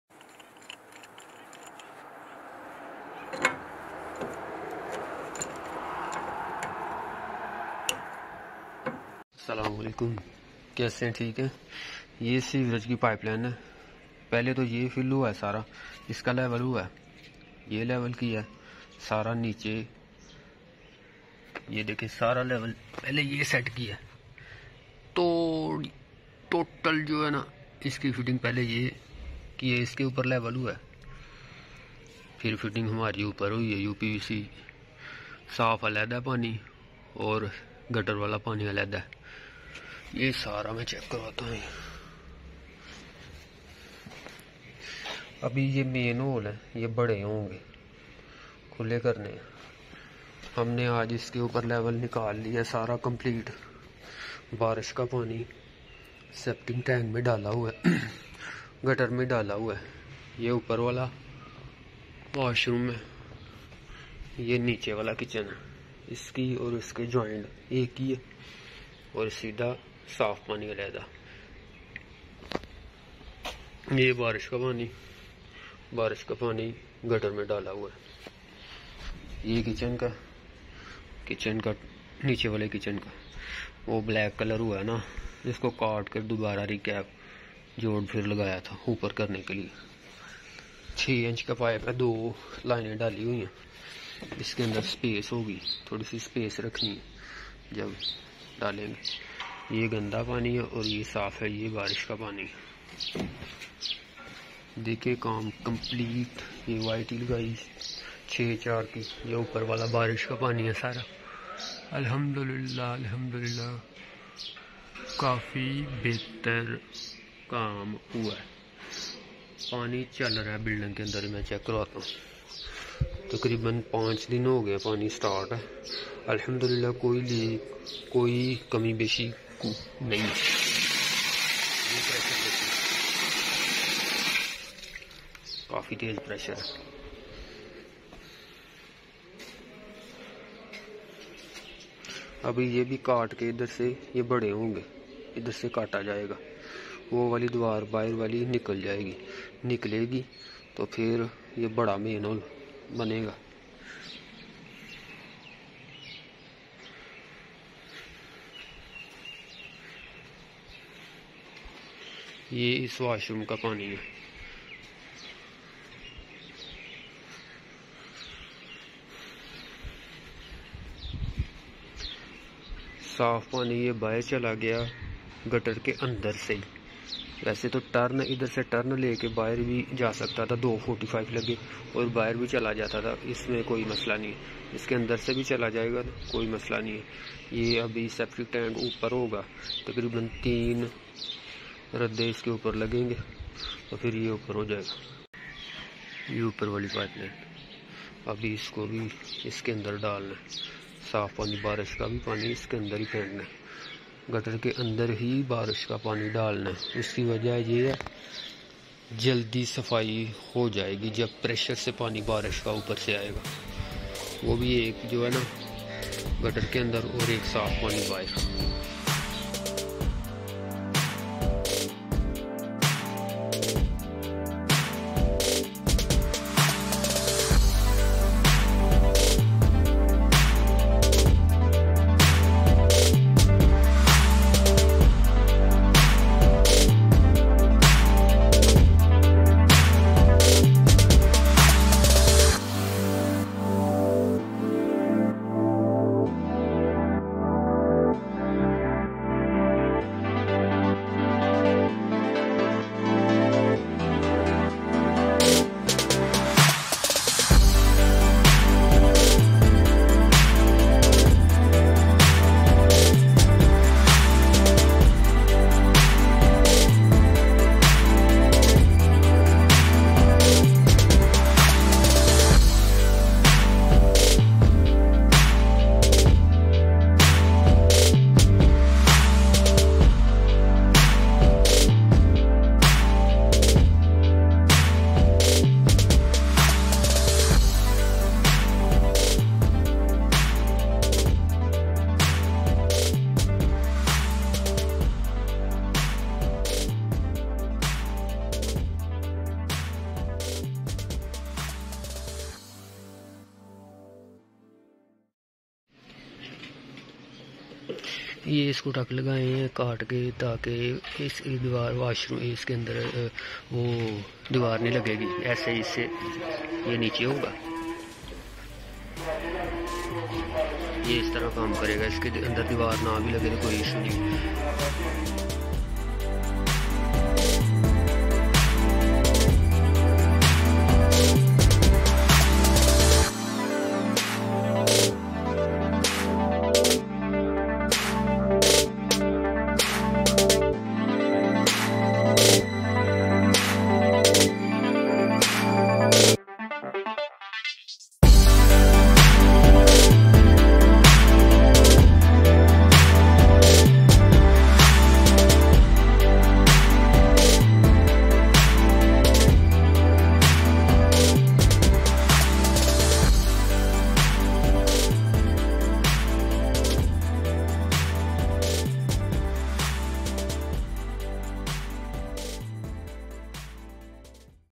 Home 🏠 pipe line settings size 6" and 4" //plumber Usman How to work mean pipe line adjustment
Expert plumber Usman
pipeline mean